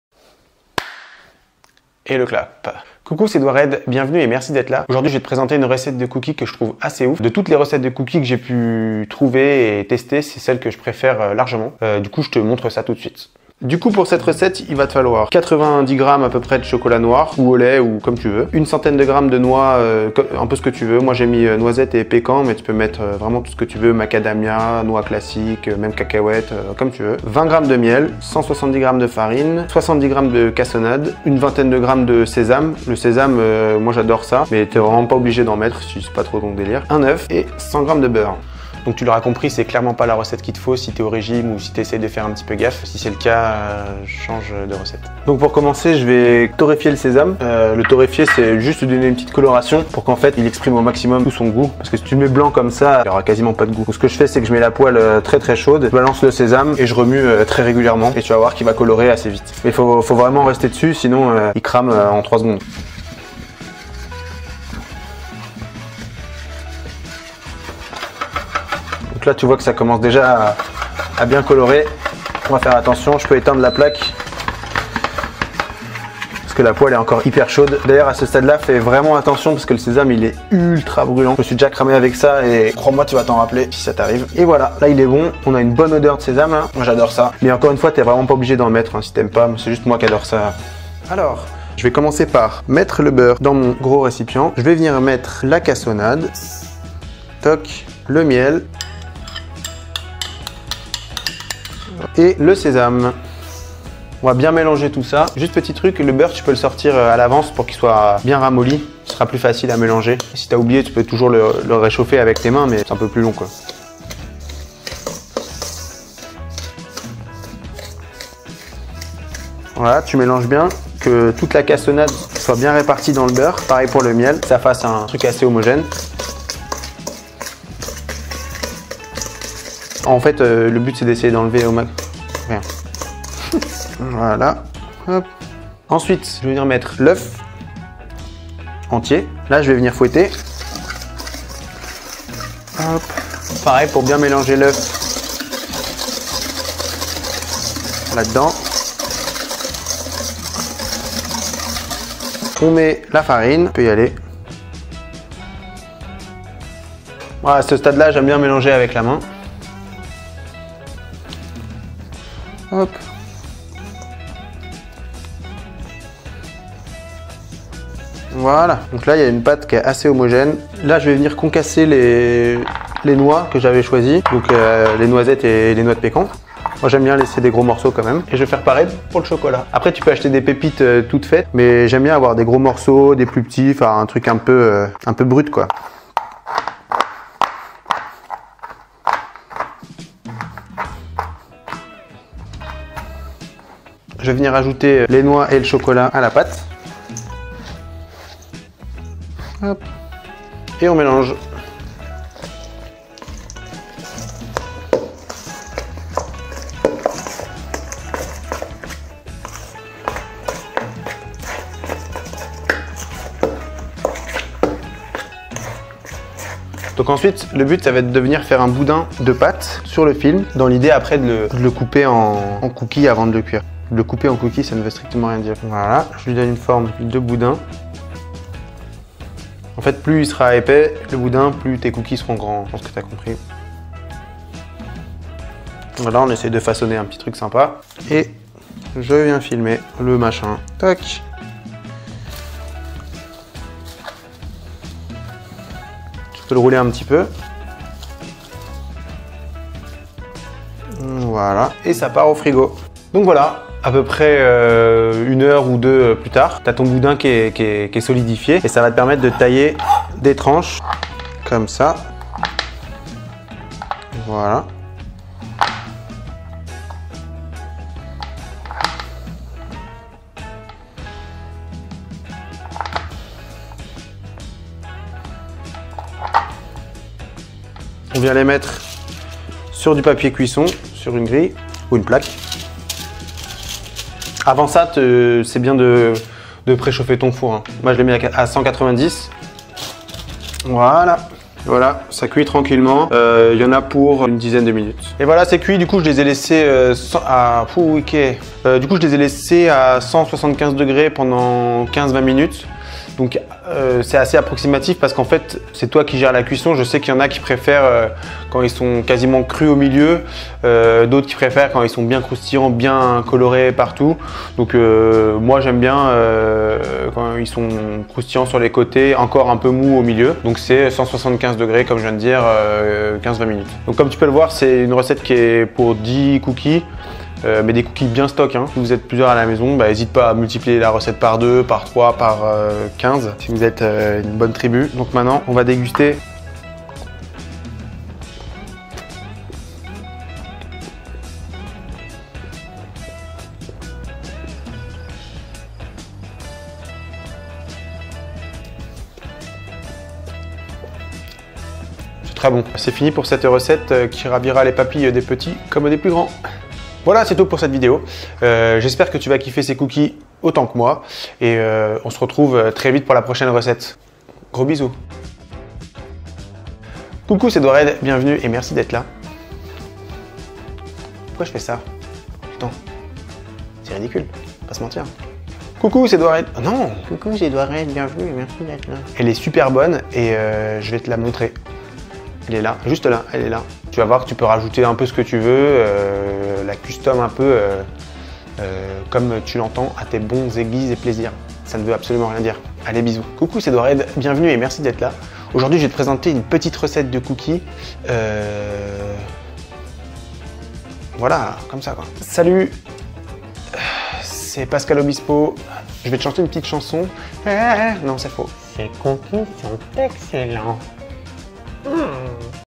Recette de Cookies de l'espace !
Yo ! Aujourd'hui je te présente ma recette de cookies favorite.
Tu peux garder la recette de base de la pâte et ajouter tout ce que tu veux :)